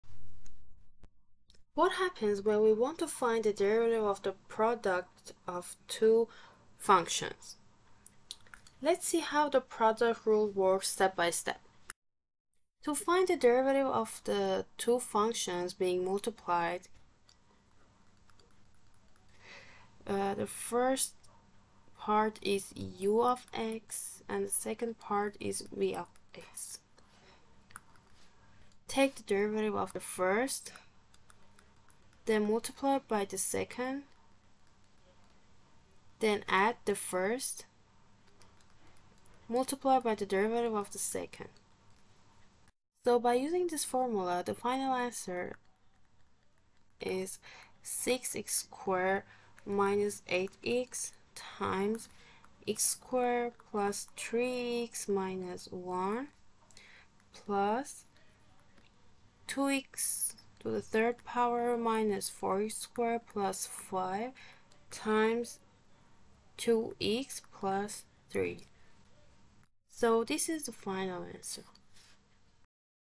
How to Differentiate Products of Functions | Product Rule Explained
In this video, you’ll learn how to find the derivative of the product of two functions using the Product Rule.